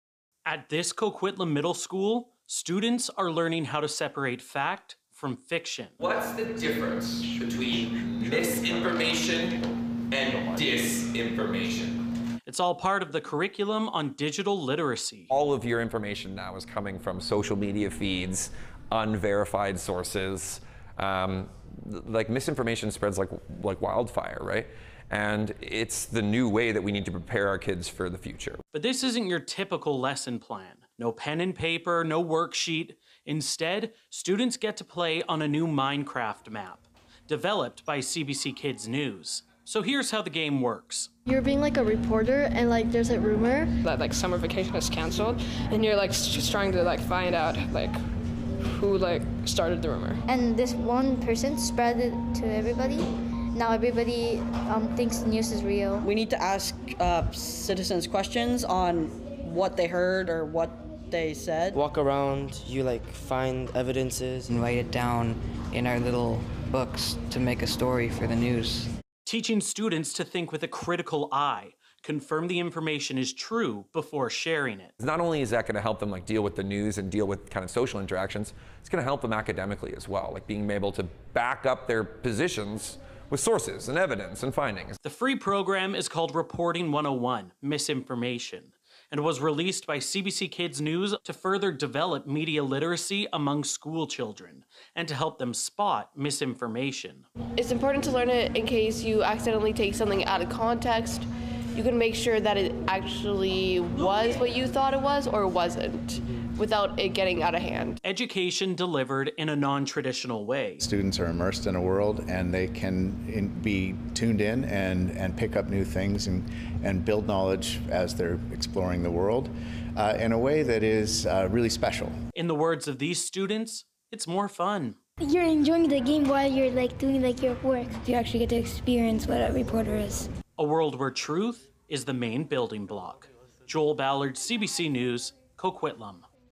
Tackling misinformation with Minecraft
As kids venture online at younger ages, teaching them about media literacy has become increasingly important. CBC Kids News released a Minecraft map that lets students explore a world to investigate a rumour. Joel Ballard visited a Coquitlam classroom to learn how, exactly, it's being used.